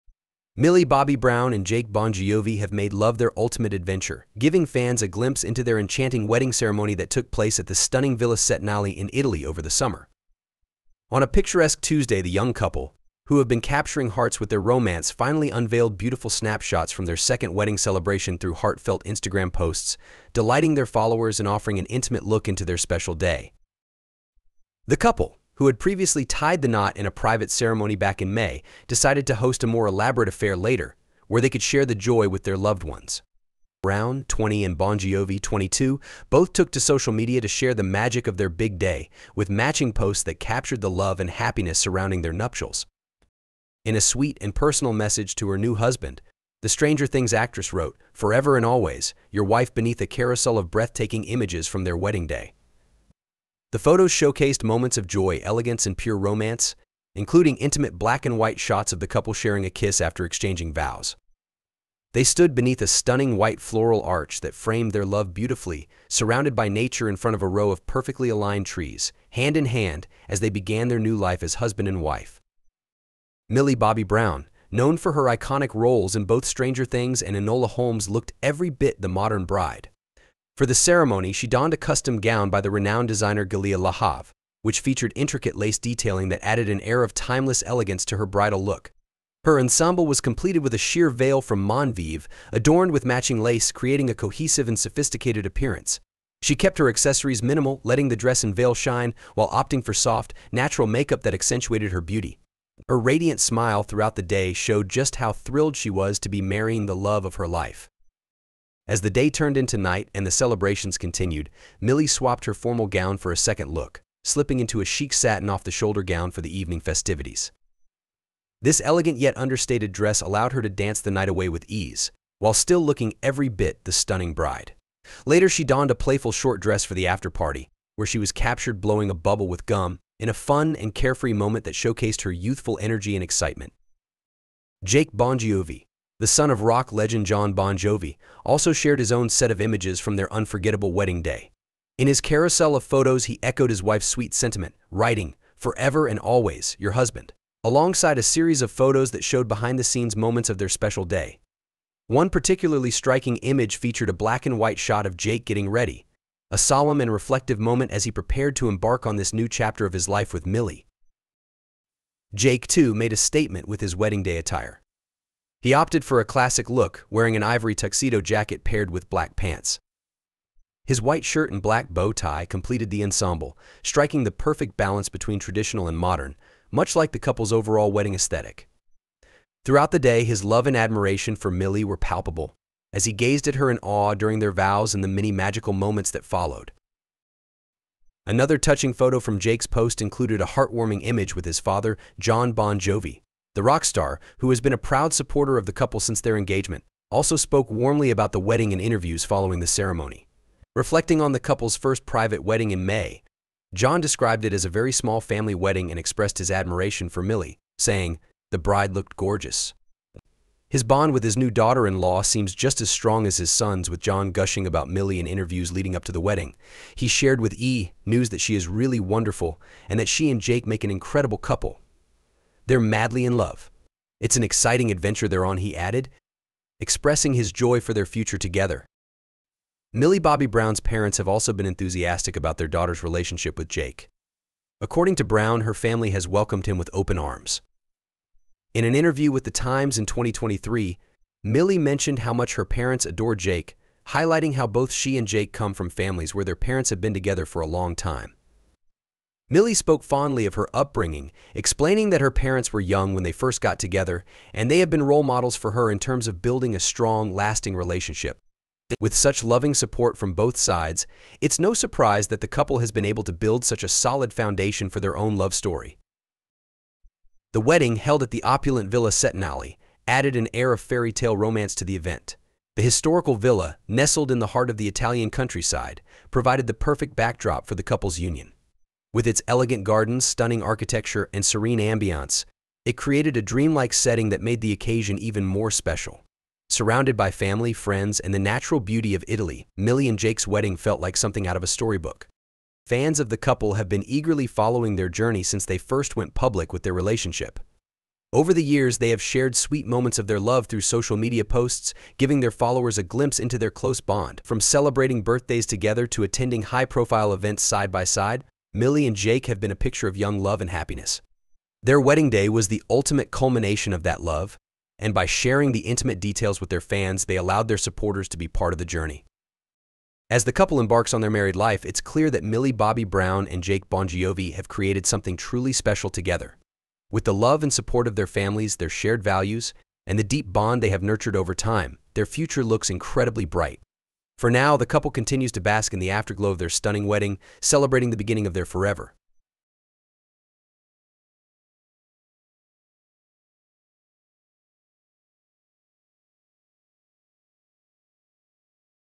Millie Bobby Brown and Jake Bongiovi Tie the Knot in a Dreamy Italian Wedding
Millie Bobby Brown and Jake Bongiovi share intimate moments from their enchanting wedding ceremony at Villa Cetinale in Italy. After secretly tying the knot in May, the couple unveiled stunning photos of their second nuptials, showcasing breathtaking gowns, heartfelt vows, and the support of their families. Discover the details of their love story and their beautiful celebration of romance and commitment.